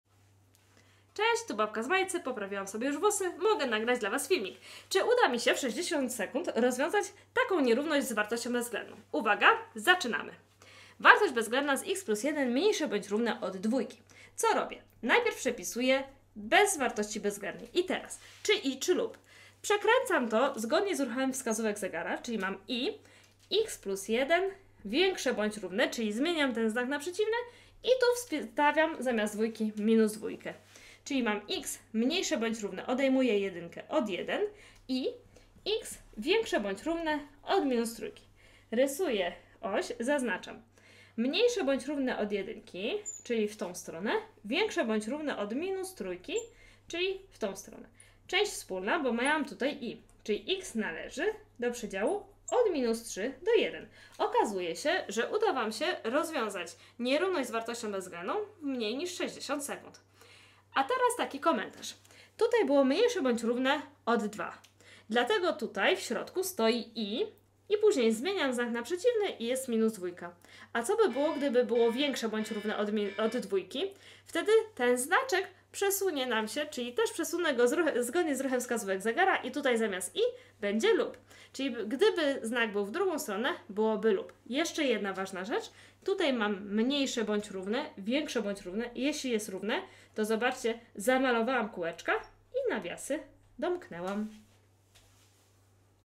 Nierówność z wartością bezwzględną w 60 sekund #8
Minuta na nierówność z wartością bezwzględną... Challenge accepted! Babka z Majcy zrobi to w 60 sekund.